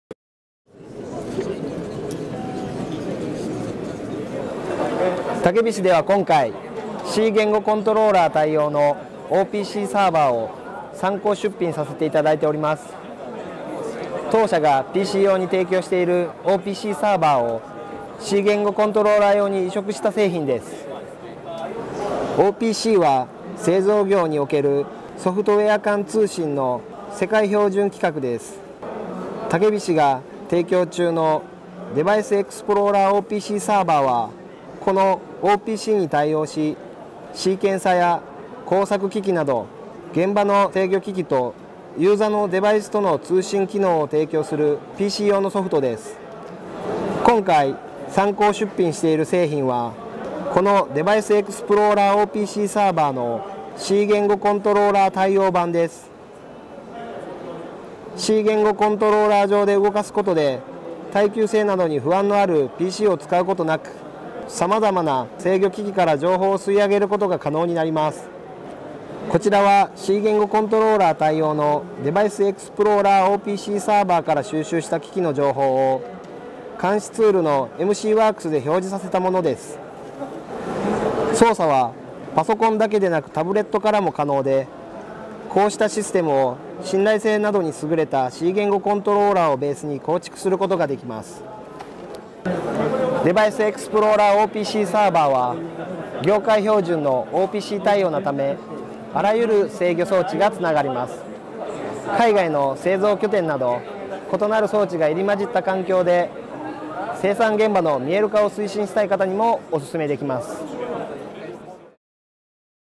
OPC server for the C controller / C言語コントローラ対応OPCサーバー 株式会社たけびし ((English)/日本語)
OPC（OLE for Process Control）サーバー機能（OPC UAインターフェイス）をC言語コントローラ上で動作させ、タブレット端末や上位サービスからMELSECと直接接続できます。
C言語コントローラをゲートウェイとして活用することで、パソコンを使用せずにICTおよびIoTシステムを構築できます。海外の製造拠点など異なる装置が入り混じった環境で生産現場の見える化を推進したい方にお勧めします。
The OPC (OLE for Process Control) server function (OPC UA interface) can be operated on C controllers and directly connected to MELSEC from tablets or upper-level service.
The ICT and IoT system can be established without a personal computer by using the C controller as a gateway.
We recommend this server to those who would like to visualize production sites in the environment where various devices are mixed such as oversea production sites.